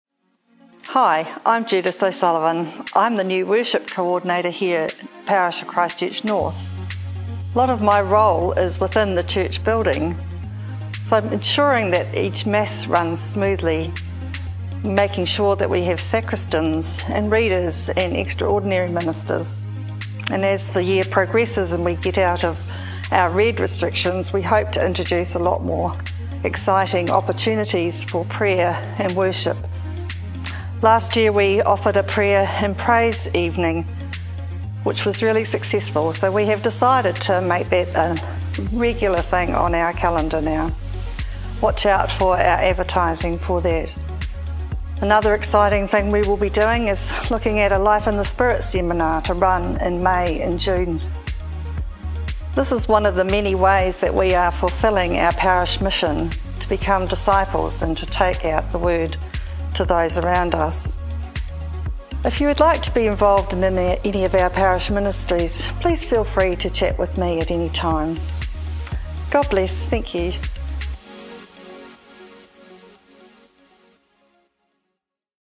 Worship | Judith O'Sullivan
Judith, our Worship Coordinator, updates our parish on what she has done, and what she hopes to achieve.

Please keep the North Parish in your prayers as we live out our parish vision.